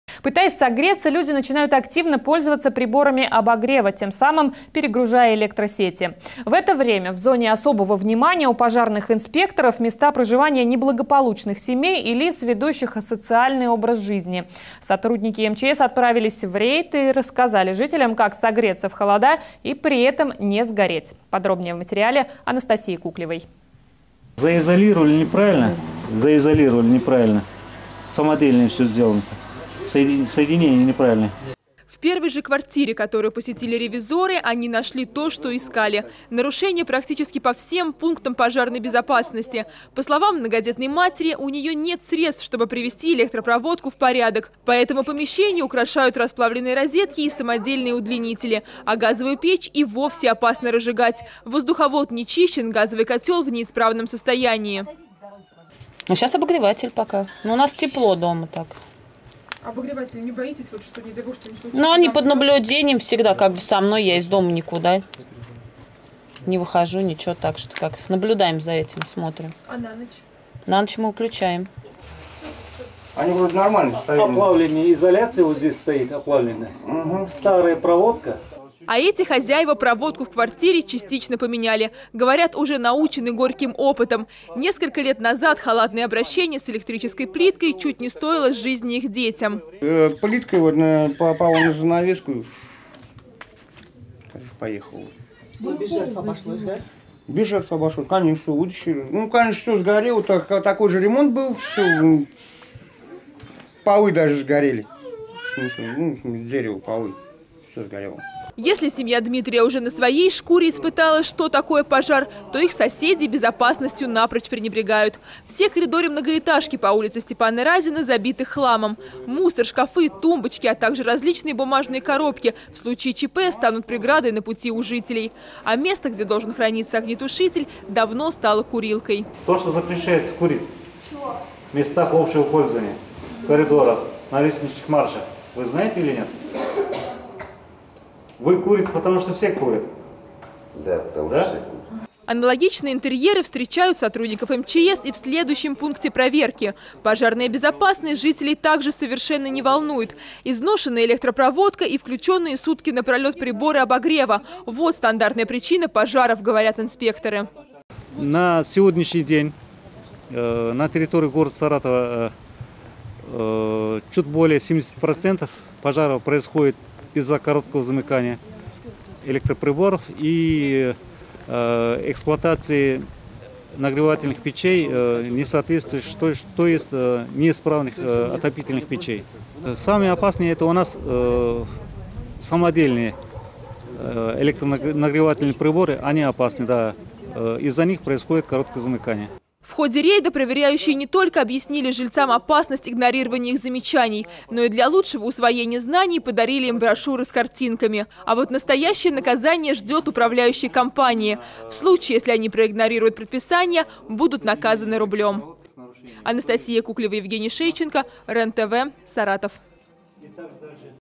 Как согреться от холода в Саратове и не сгореть?
Пытаясь согреться, люди начинают активно пользоваться приборами обогрева, тем самым перегружая электросети. В это время в зоне особого внимания у пожарных инспекторов - места проживания неблагополучных семей и лиц, ведущих асоциальный образ жизни. Сотрудники МЧС отправились в рейд и рассказали жителям, как согреться в холода и при этом не сгореть. Подробнее в материале Анастасии Куклевой от 9 октября 2015 года.